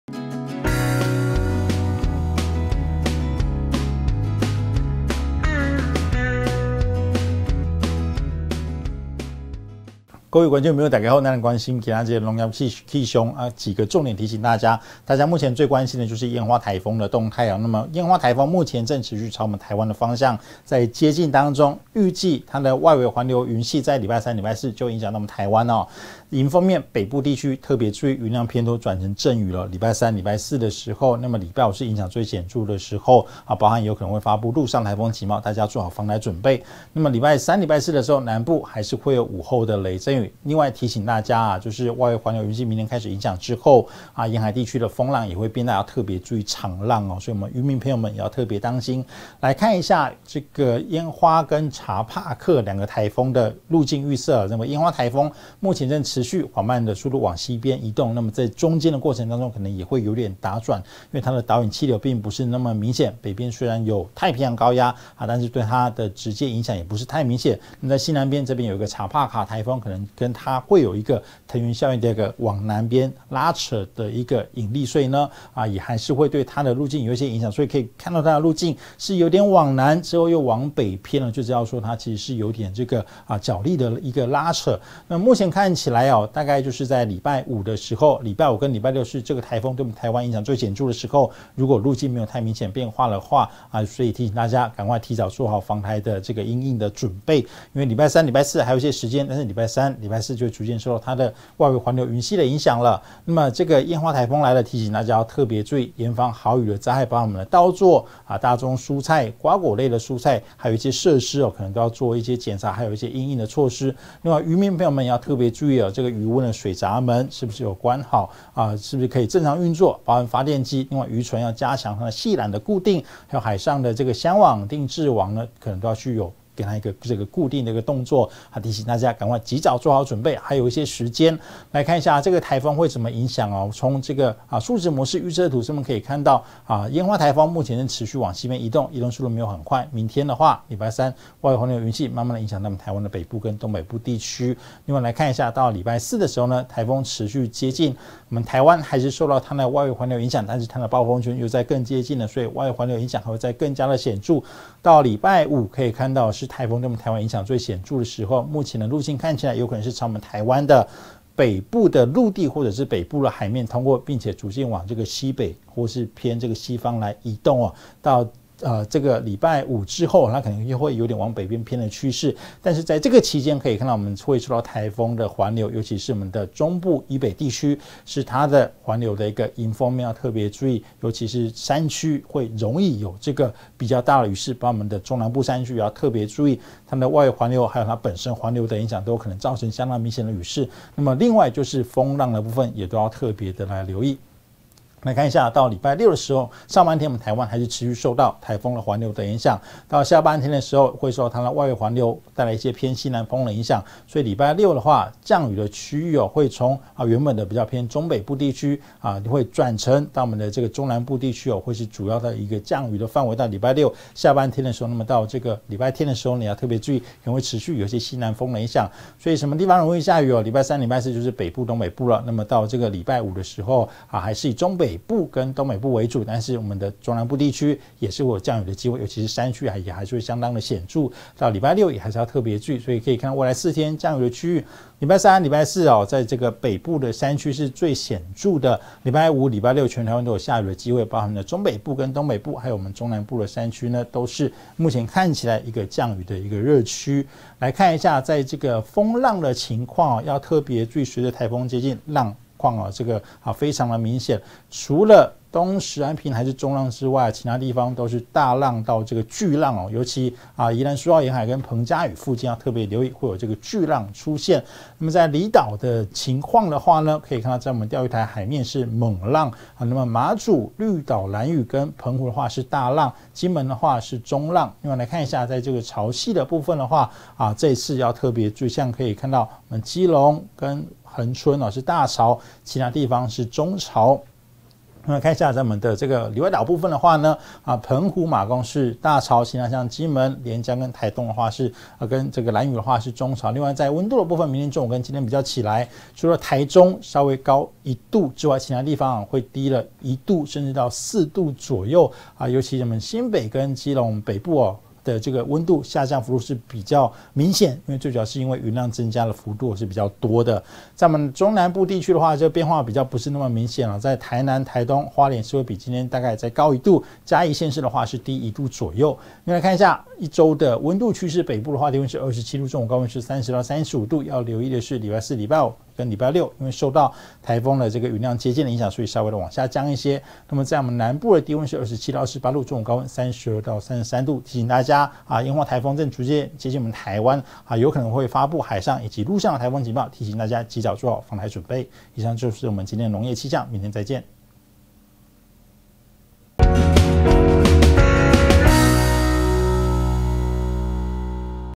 農業氣象 110/07/21 烟花颱風外圍影響，北部多雲有陣雨
颱風外圍偏北風影響。
北部多雲轉陣雨。
南部仍有午後雷陣雨。
沿海風浪轉大，需留意。